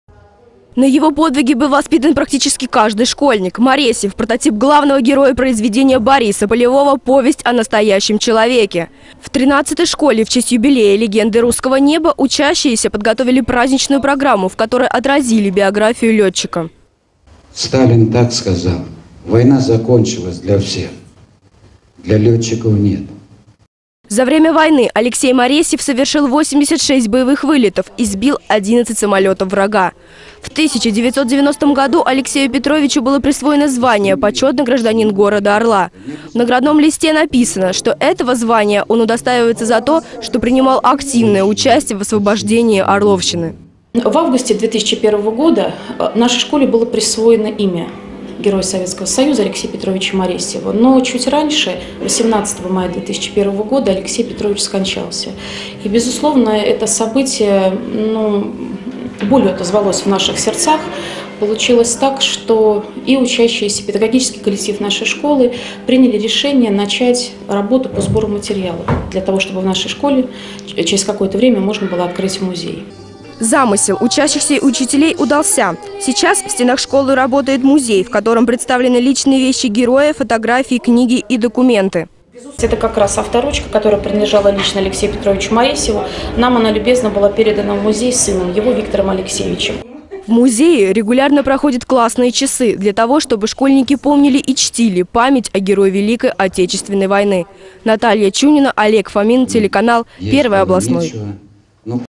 100 лет со дня рождения Маресьева
В Орле отметили 100 лет, со дня рождения героя Советского Союза Алексея Маресьева. Легендарный летчик, который несмотря на тяжелую травму не оставил мечту о небе.